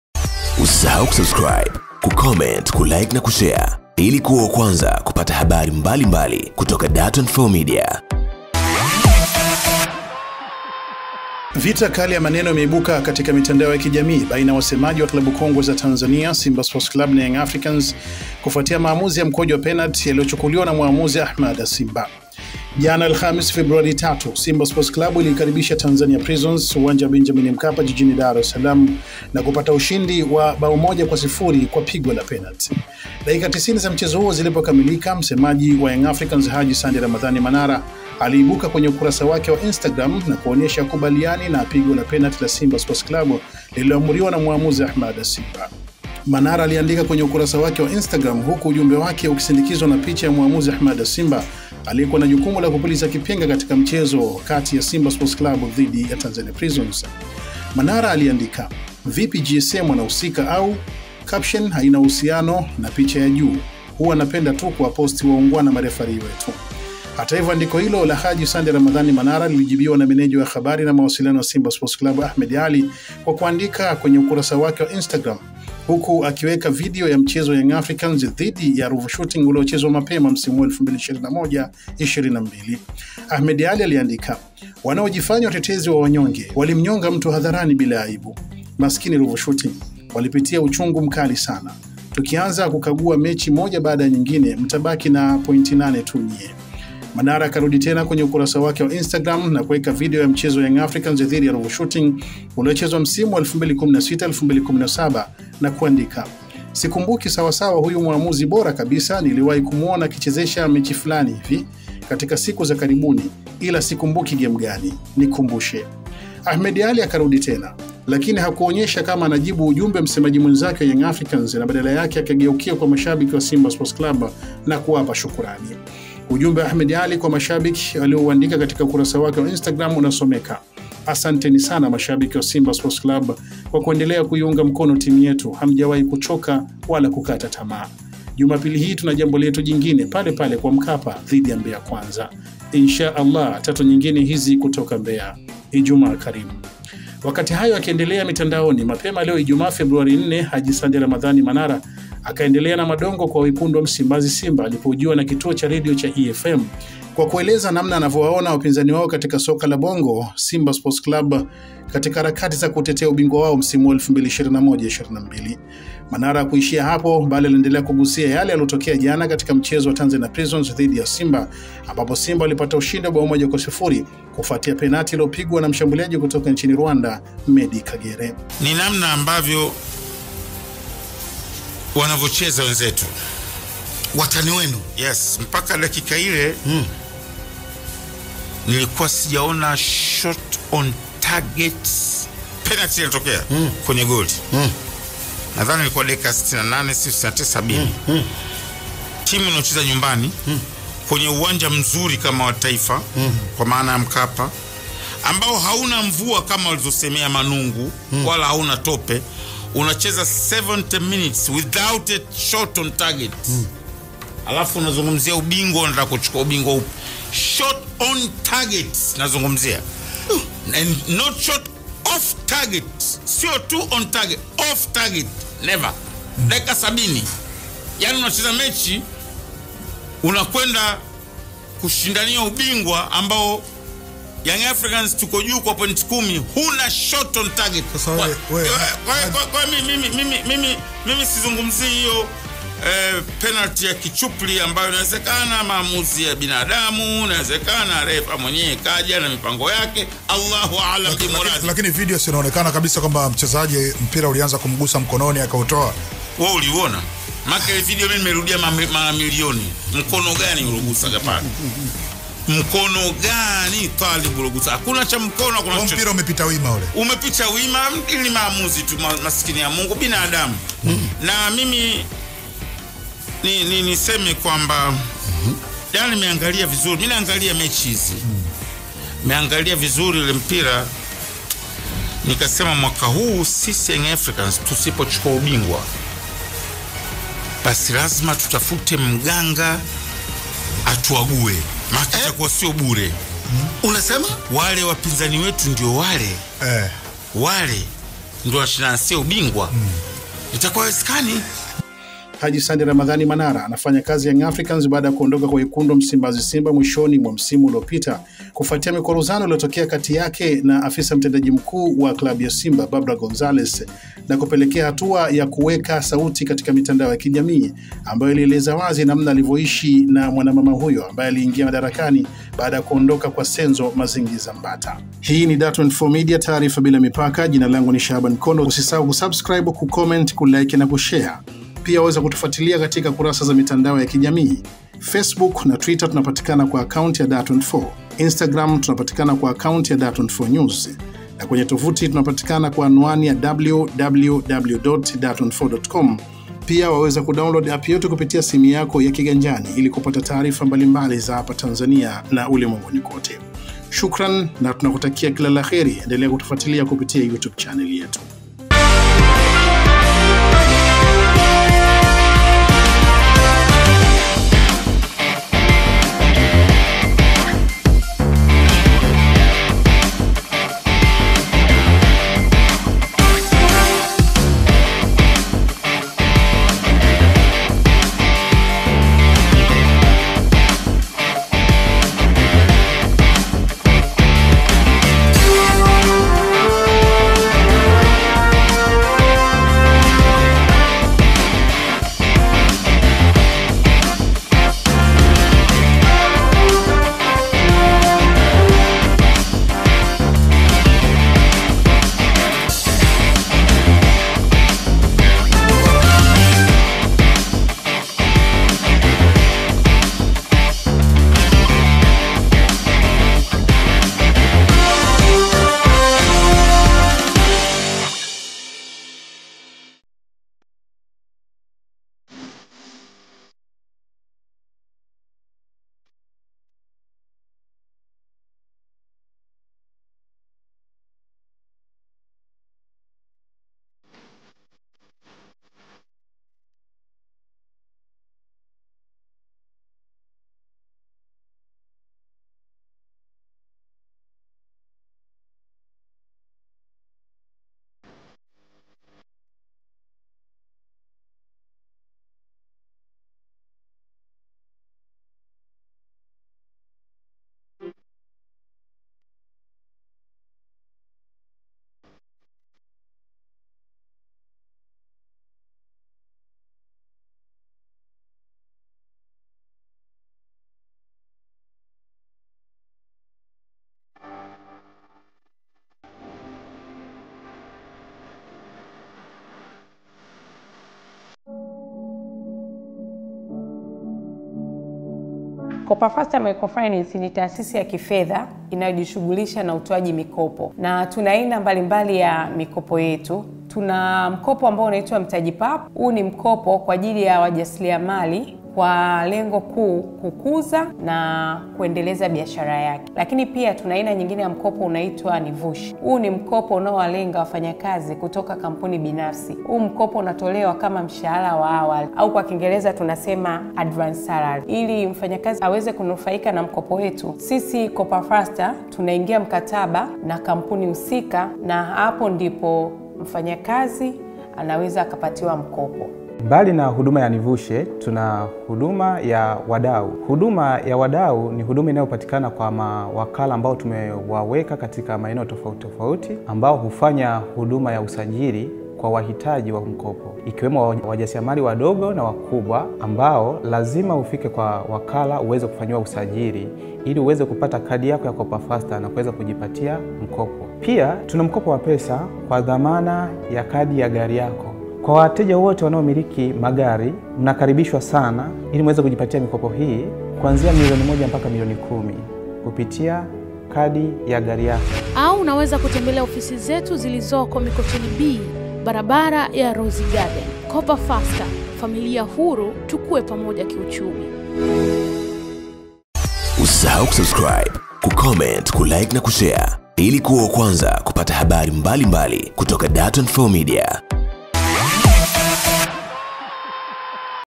YAMELIPUKA!! YANGA KWENDA KWA WAGANGA/ USHAHIDI WAANIKWA/ MANARA AUKANA!
VITA YA UBINGWA WA TANZANIA BARA IMEFIKIA PABAYA, KUFUATIA SIRI ZA KLABU KUBWA NCHINI KUANZA KUFICHUKA. YANGA WATAJA MAMBO YA WAGANGA, SIMBA WAANIKA USHAHIDI